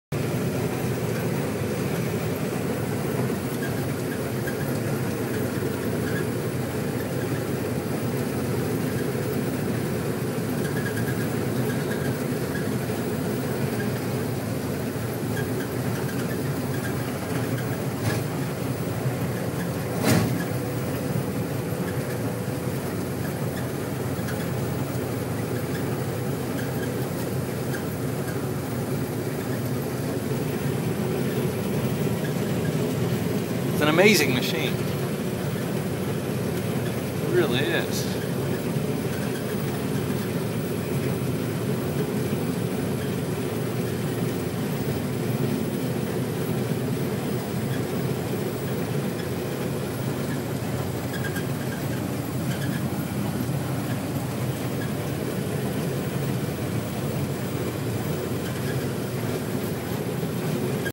Driving the combine
Harvesting wheat in the UK. A view from the drivers seat as the combine takes it in and processes the wheat